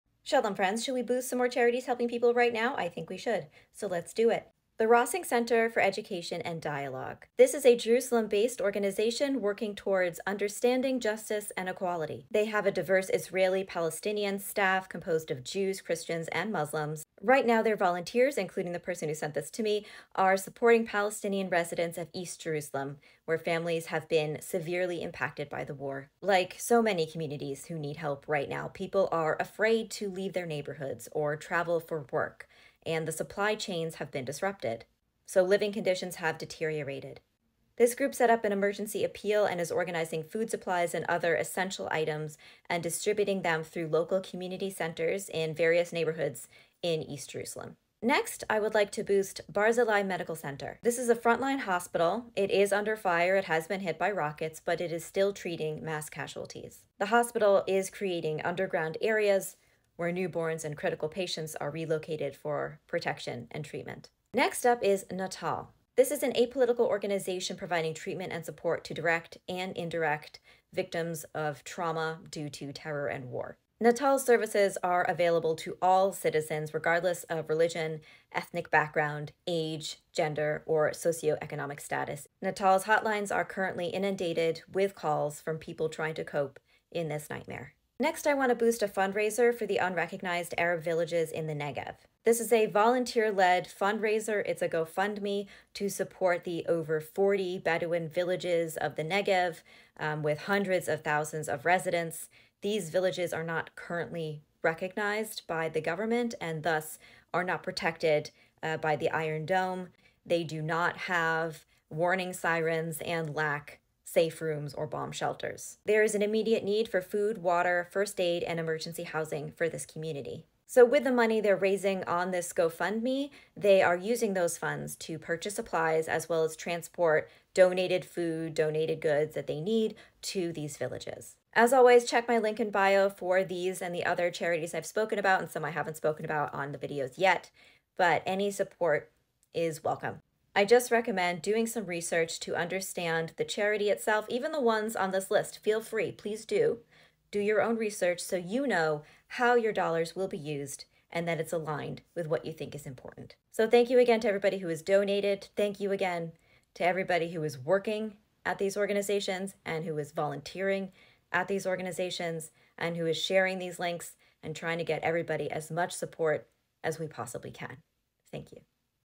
Israeli & Palestinian Aid Boost #3
Shalom friends, today let’s boost 4 more ways to help right now:
The Rossing Center, Natal Trauma Center, Barzilai hospital, and a fundraiser for the unrecognized Bedouin villages of the Negev.